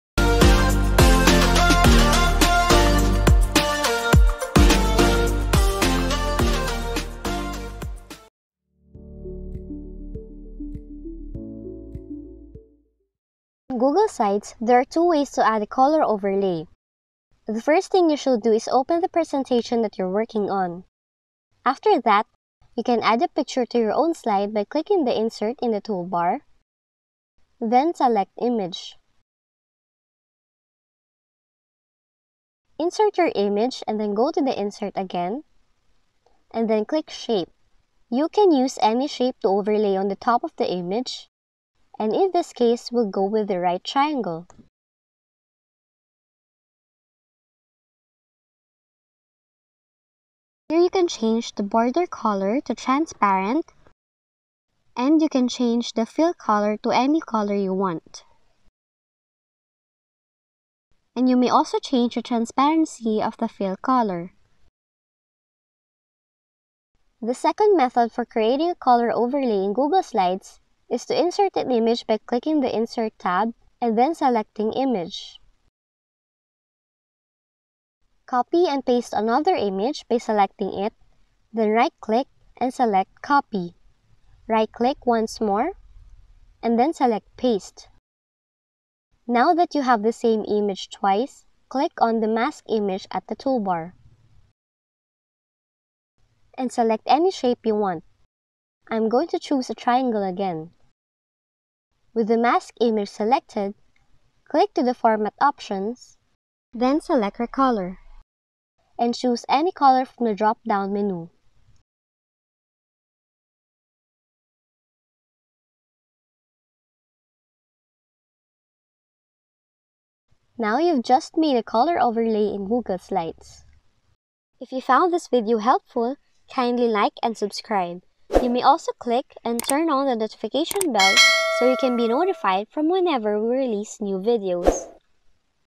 [TUTORIAL] How to (Easily) Create a COLOR OVERLAY in Google Slides
--- You can easily overlay any color on top of any image that you choose in Google Slides.  Check out this short video for 2 easy methods. ---